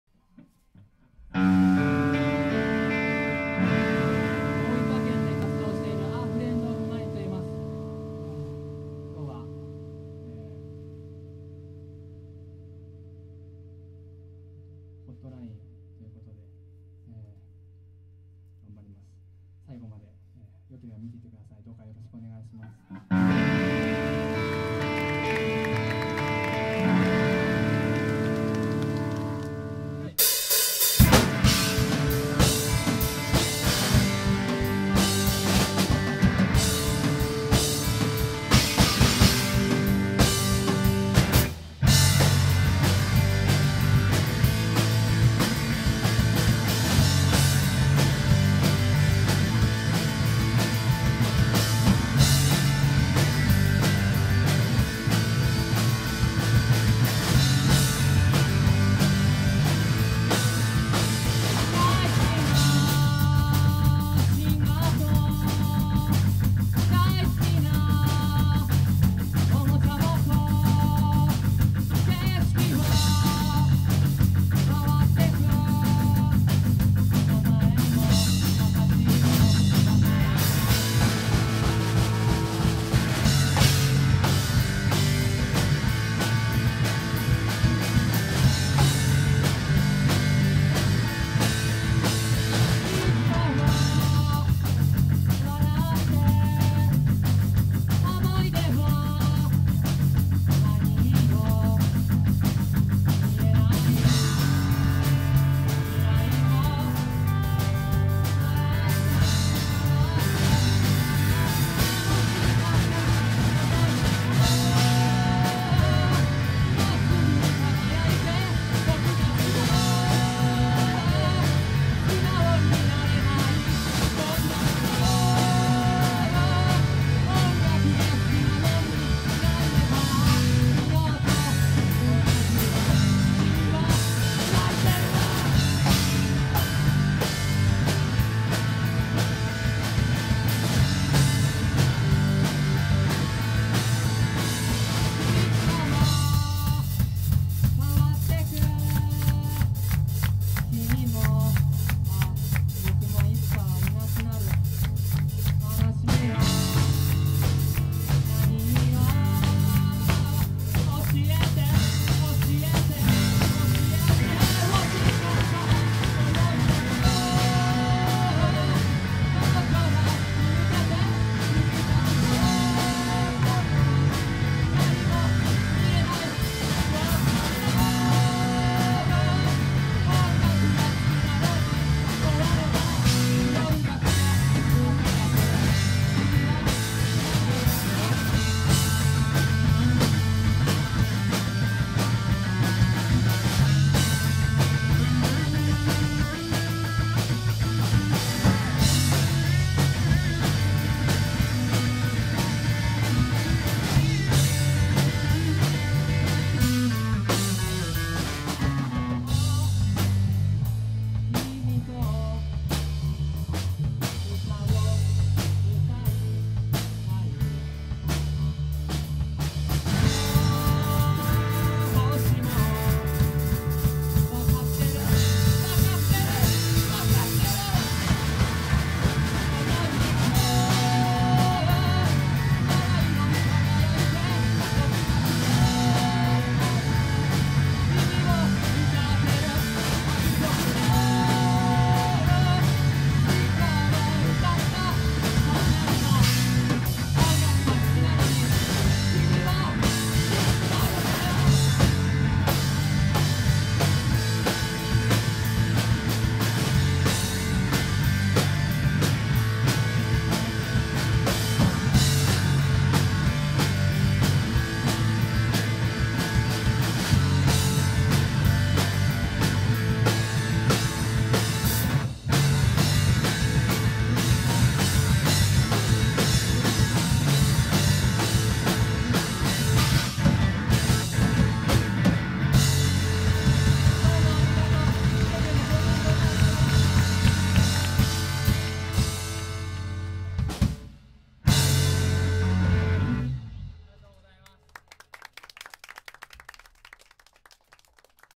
a friend of mine HOTLINE2014店予選会 大分店
島村楽器大分フォーラス店で8月3日に開催された、HOTLINE2014 店予選のレポート動画です。